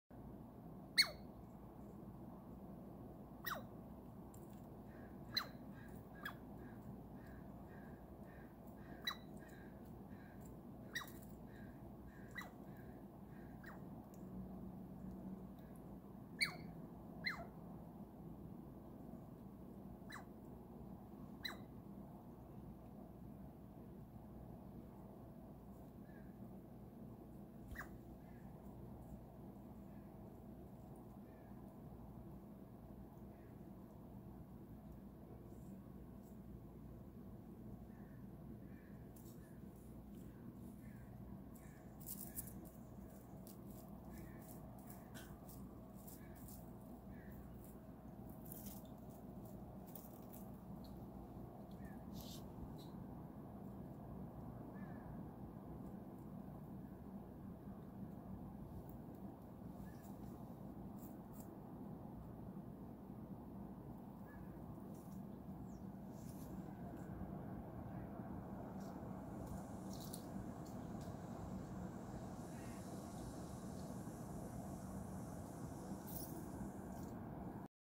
Baby ninja squirrel starting the day - yawning on the tree
yawning at 0:32